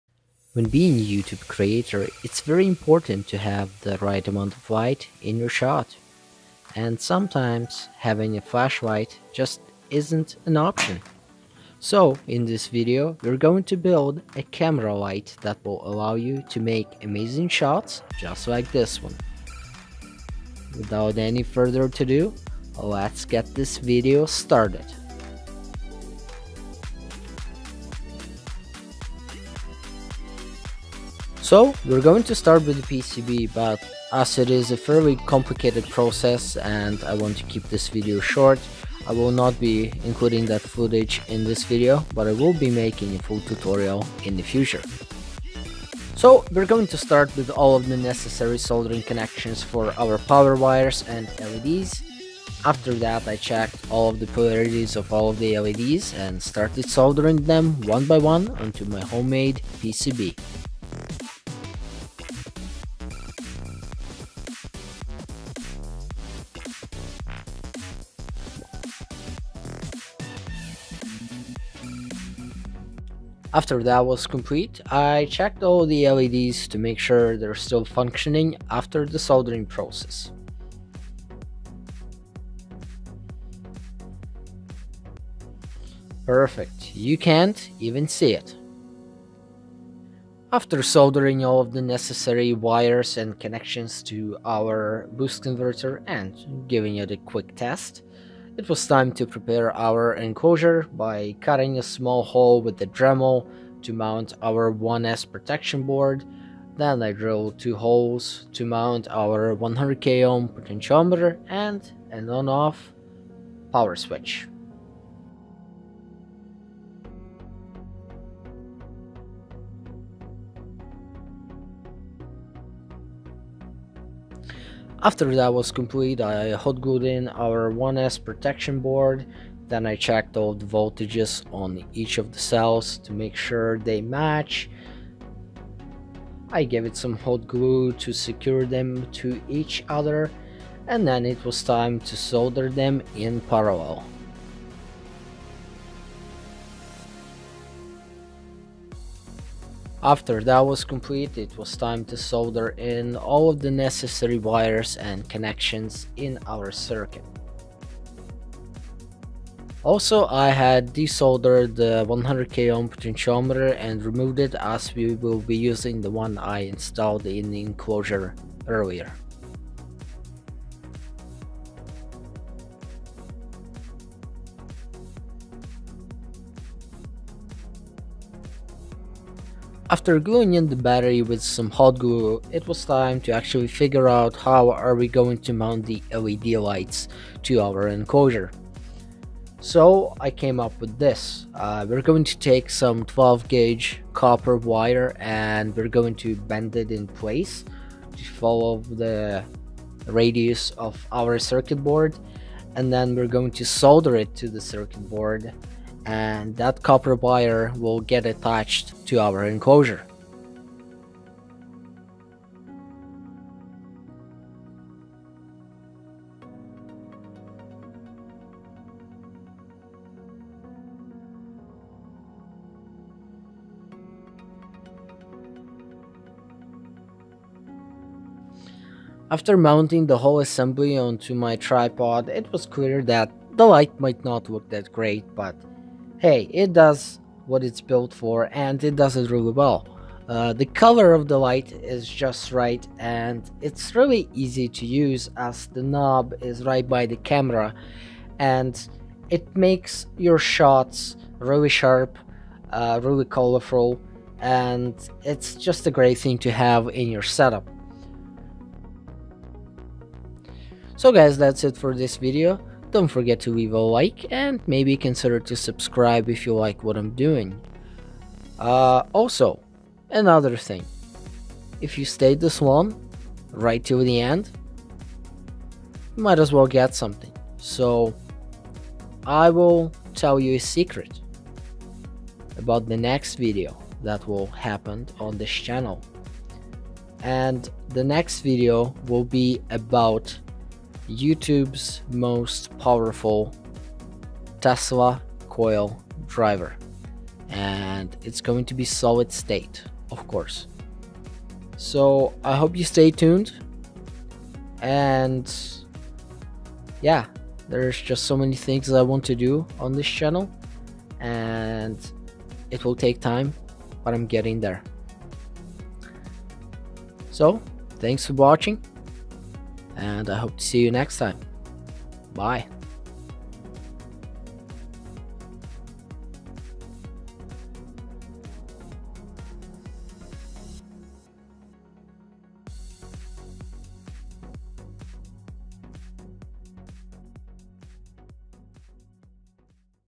DIY CAMERA LIGHT TUTORIAL
WELCOME TO EEC!
In this video we have made a DIY super bright camera light!!!
It works really well and i will be using it in in future videos!!
THIS IS A PLACE FOR MOST COOL PROJECTS ON YOUTUBE